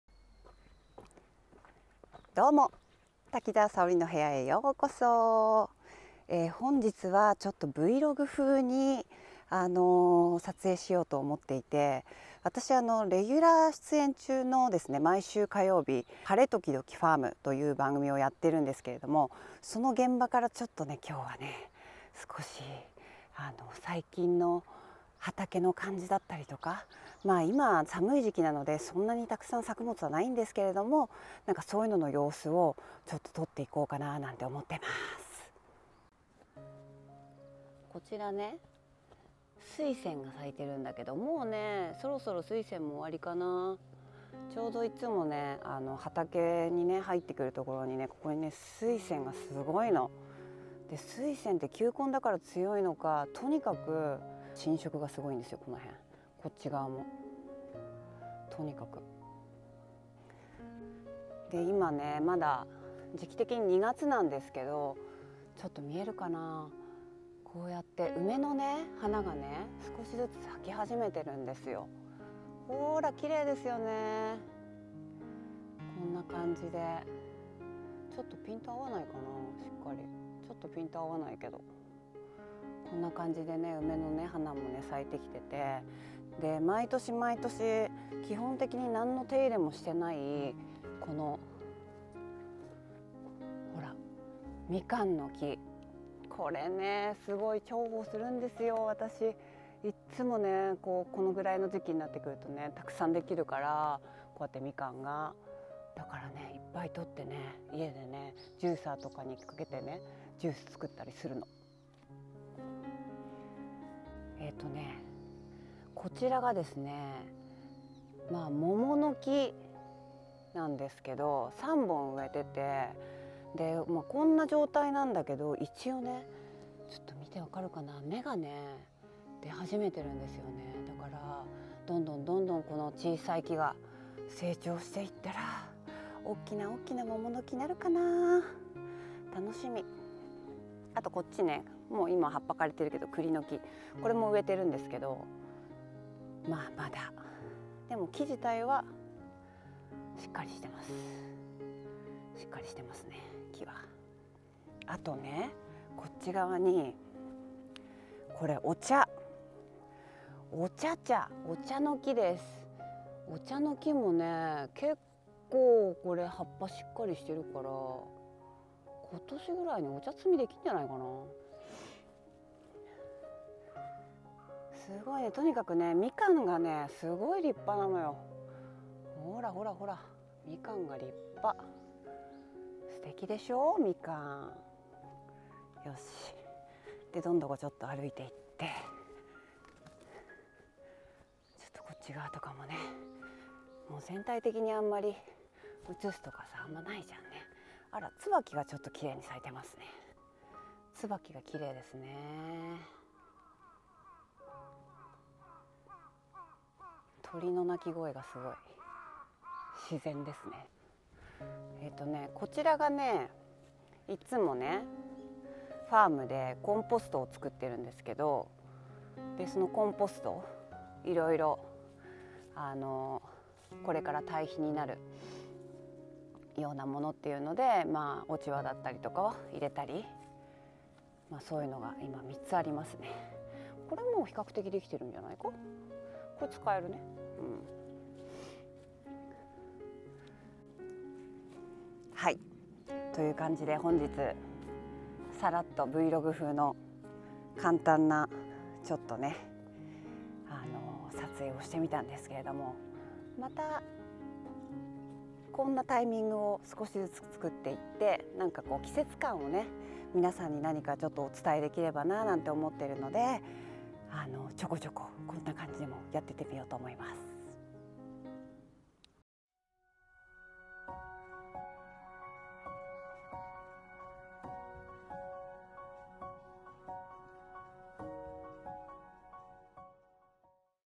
【Vlog】四季を感じる レギュラー番組『晴れ、ときどきファーム！』の現場をレポート
今年で９年目となるBSプレミアム『晴れ、ときどきファーム！』
いつも撮影している古民家に行くと癒されます😄✨
皆様にも時々こんな感じで癒しをお届けしますね〜💗
番組も是非ご覧下さいませ❗️
☆番組のホームページはコチラ↓
『晴れ、ときどきファーム！』

★お仕事に関するお問合せは事務所のホームページよりお願い致します。